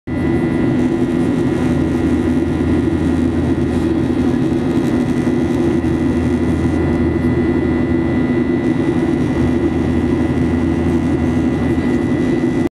Tbilisi aerial - Полёт над Тбилиси
Флайт Тбилиси-Киев 31 мая 2015. МАУ.
Цыпленок-Бройлер 737.